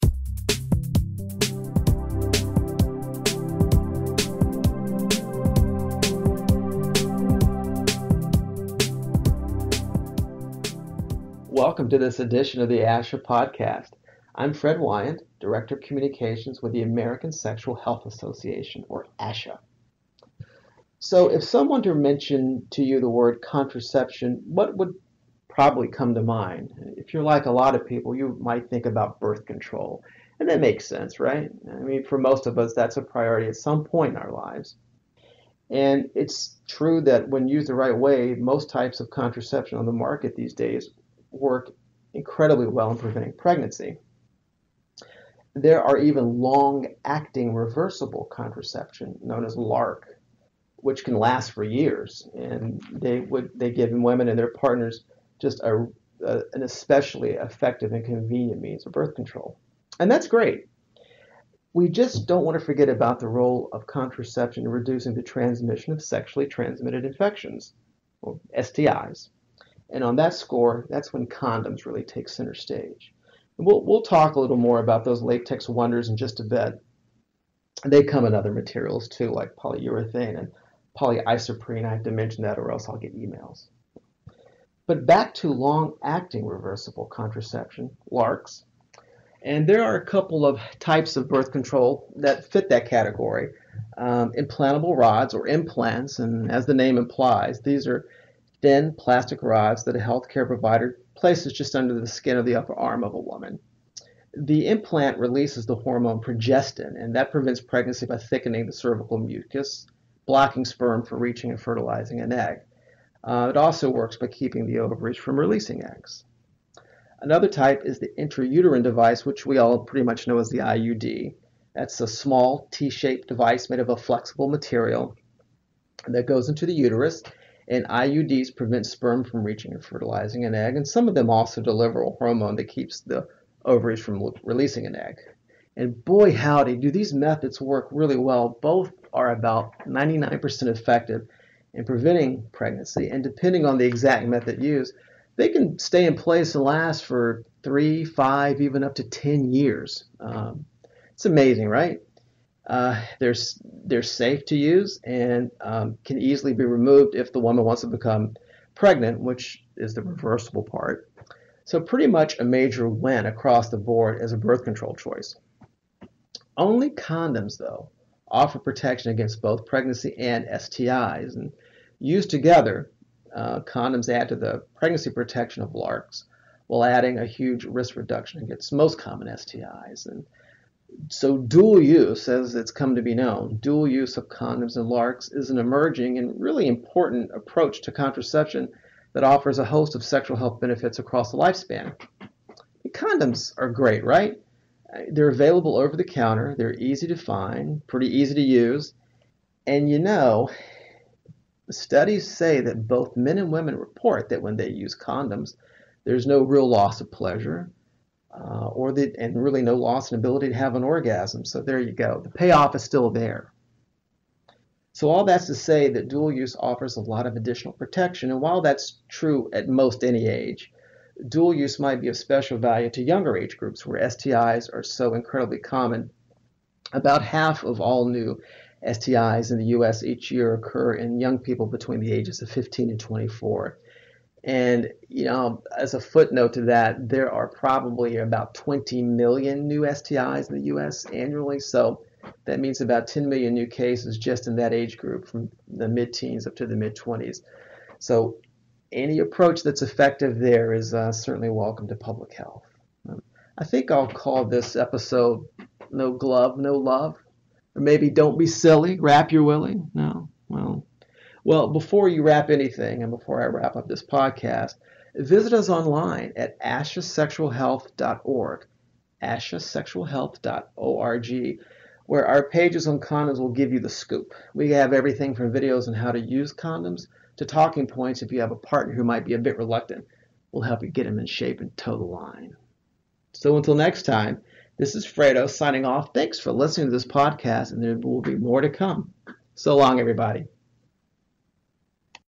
Dual Use of Condoms and LARC
Long-acting reversible contraception, or LARC, is reversible birth control that provides long-lasting (think years) pregnancy prevention. LARC methods, which include intrauterine devices (IUDs) and implants, are highly reliable, but they don't prevent STIs. For this reason, many choose to use (and health professionals recommend) condoms in addition to a LARC method. Dual use of condoms and LARC thus offers couples dual prevention. ASHA's Fred Wyand (a.k.a. Fredo) discusses dual use of LARC and condoms in the latest episode of ASHA Sex+Health podcast. Fred explains the facts about LARC and the added benefit of dual use of LARC and condoms to prevent STIs.